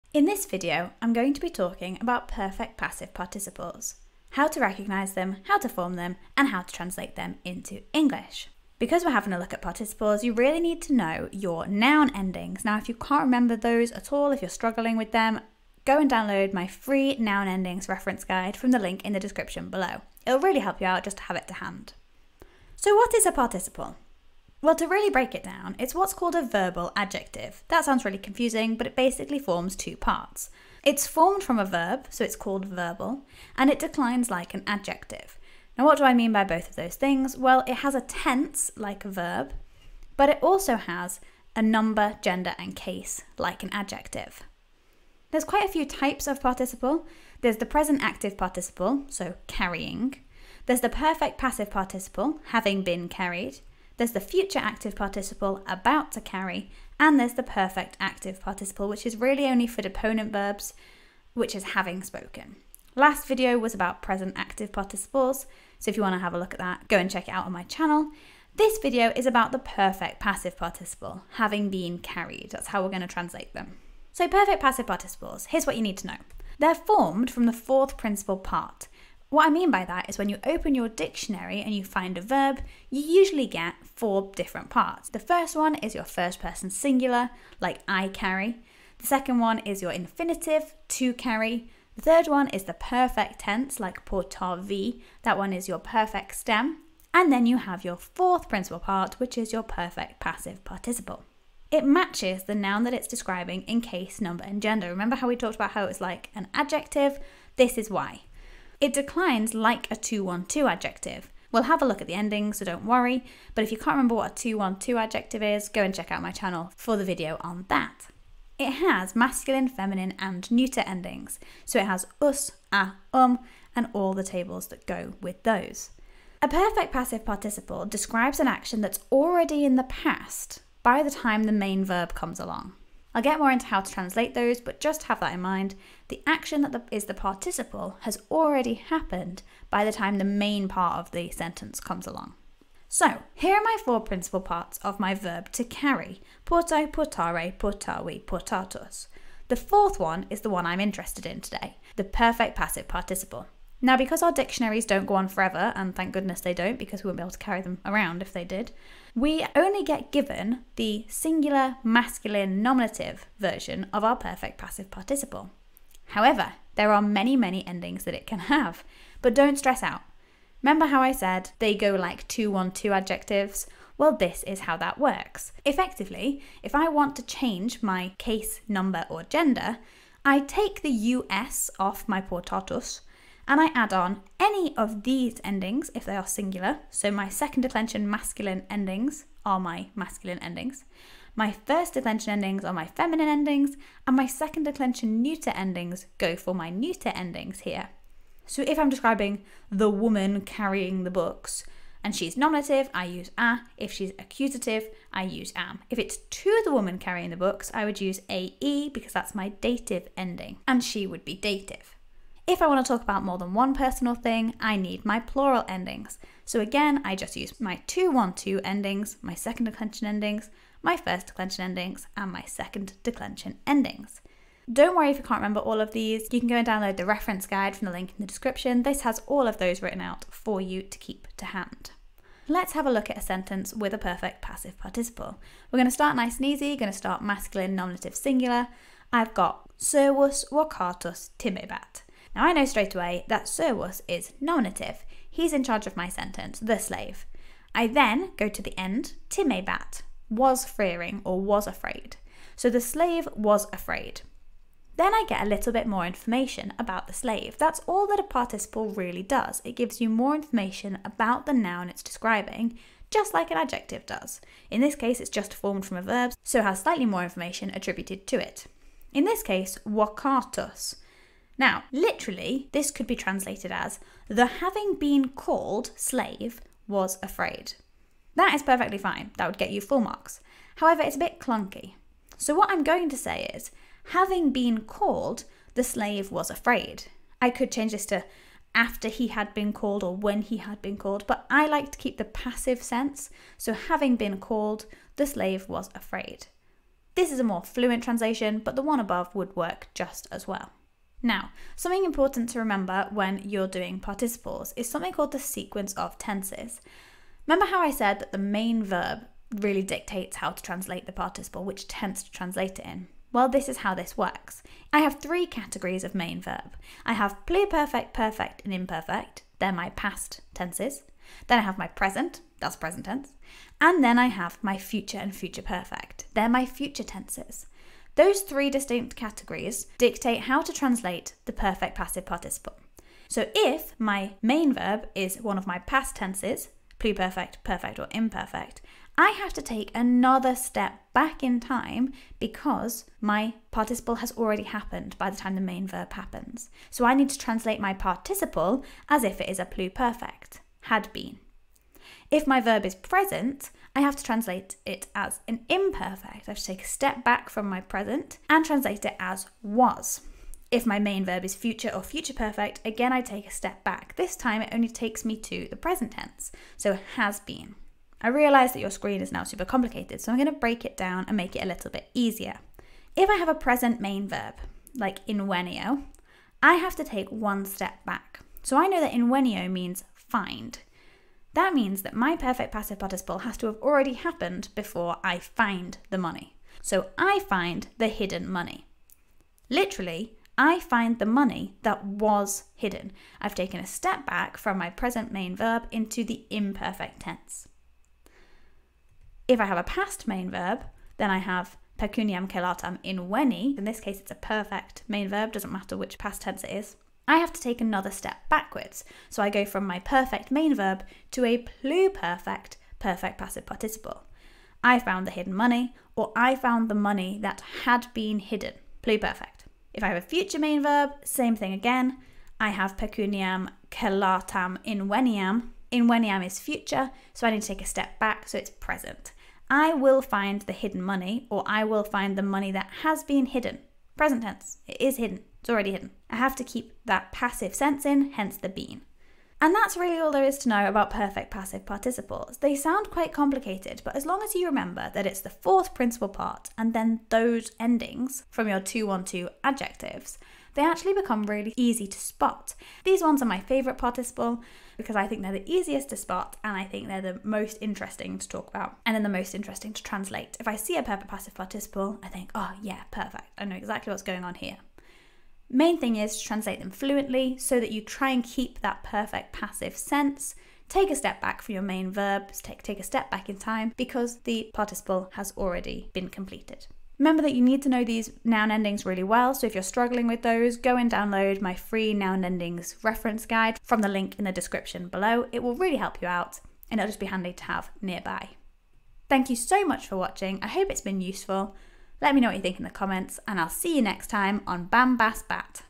Perfect Passive Participles in Latin
What are Perfect Passive Participles in Latin? How does the 4th principal part come into it? In this video I will show you how to recognise, form, and translate PPPs.

0:00  Intro
0:13  Complete Guide to Noun Endings
0:22  What is a participle?
0:46  Types of Latin Participle
1:14  How to form them
2:22  Form in a dictionary
2:55  Agreement with singular nouns
3:34  Agreement with plural nouns
3:55  Example in Latin with translation
5:01  Sequence of Tenses
6:12  Sequence of Tenses examples
7:20  Wrap up